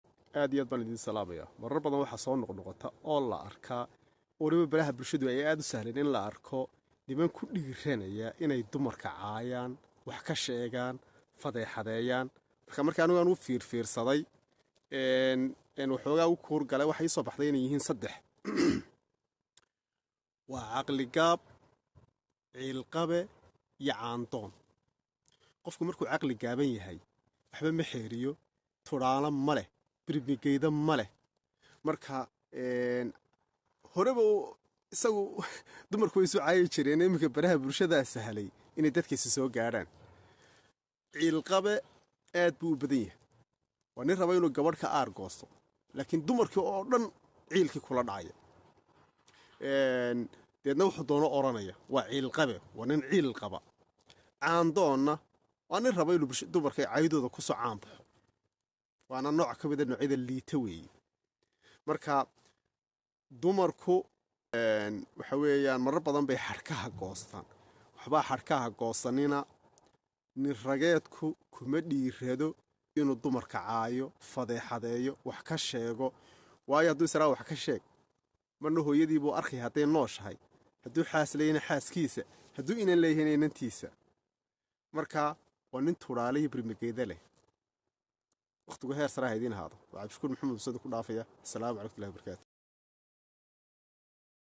Waa Saddex Ka Madhan Birmagaydada Iyo Tudhaalaha| Niman Caadaystay Fadeexadaynta Iyo Cayda Dumarka!
Waa saddex ka madhan birmagaydada iyo tudhaalaha. Waa niman caadaystay cayda haweenka oo baraha bulshada ka fadeexadeeya dumarka!  Waa: caqligaab, ciilqabe iyo caandoon.